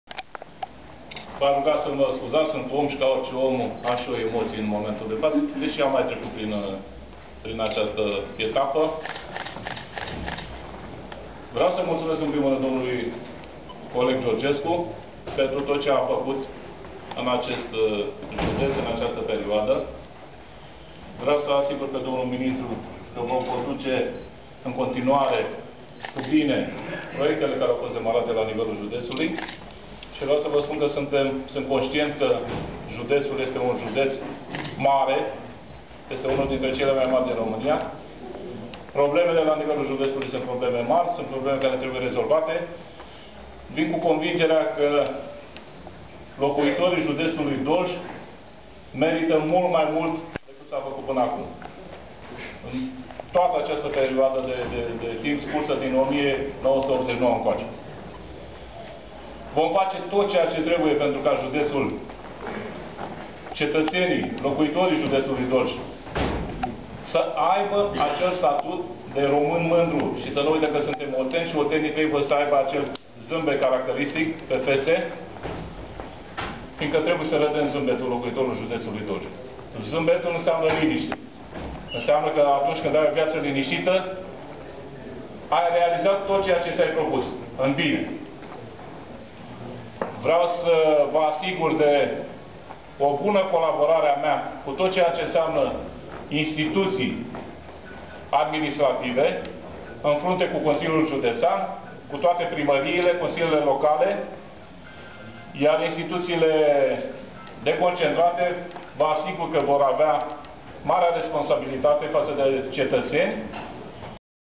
Discursul lui Nicolae Giugea la investirea in functia de prefect al judetului Dolj